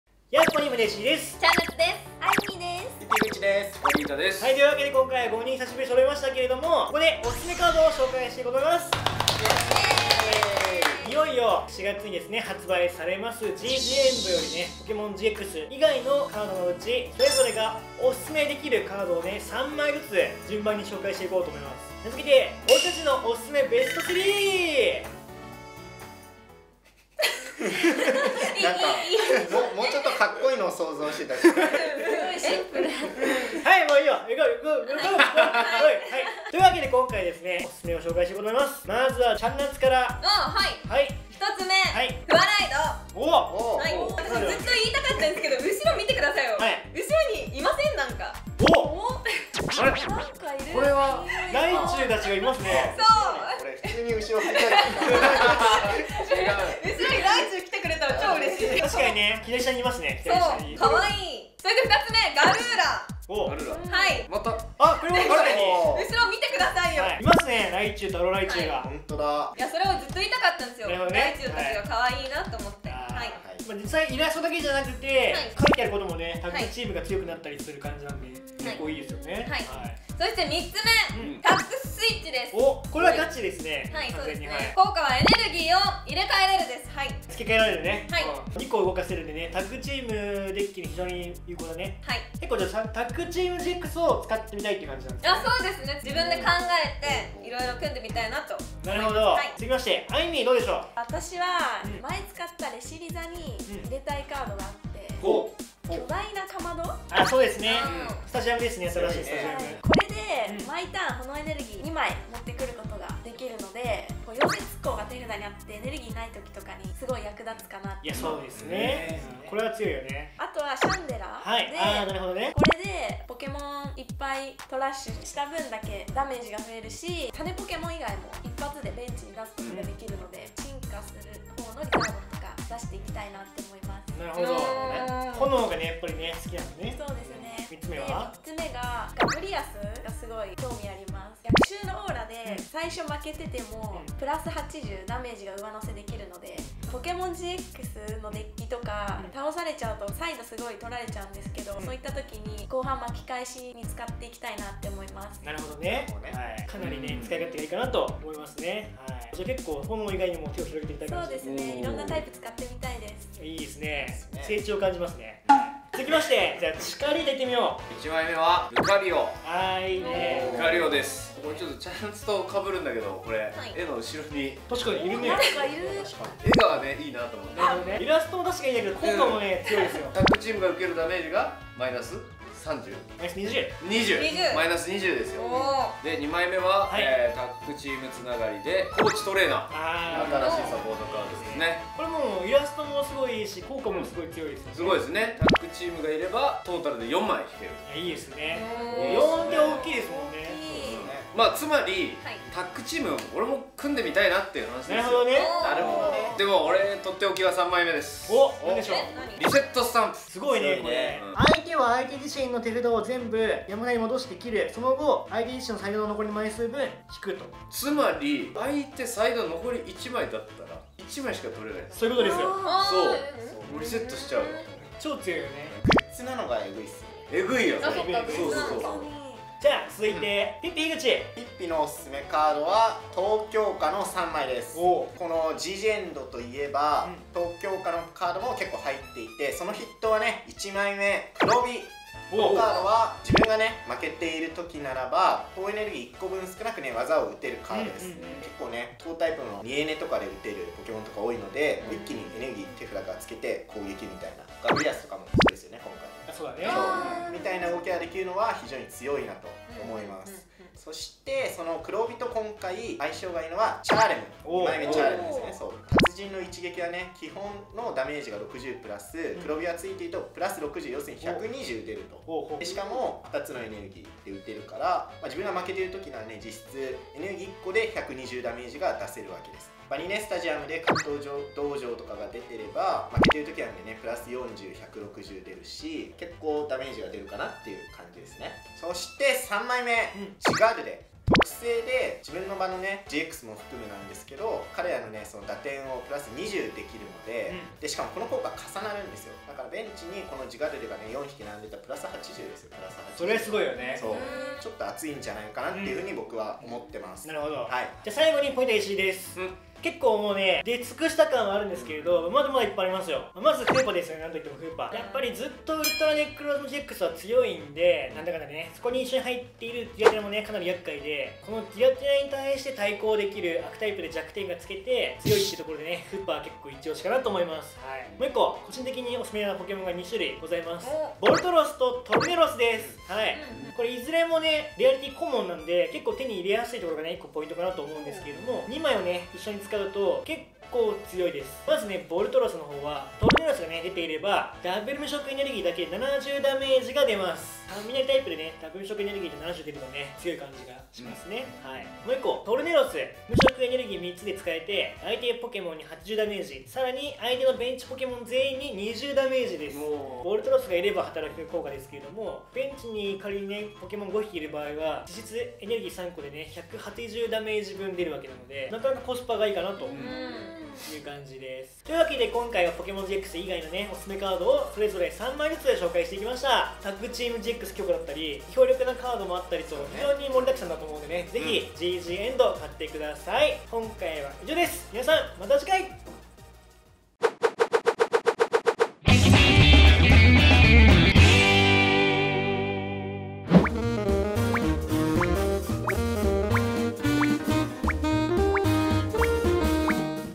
【ジージーエンド】みんなのイチオシカード15選（GX以外）
4/5発売のジージーエンドより、新カードの中からGX以外のオススメカードを５人が３枚ずつ紹介！

[ポケモンカードチャンネルとは]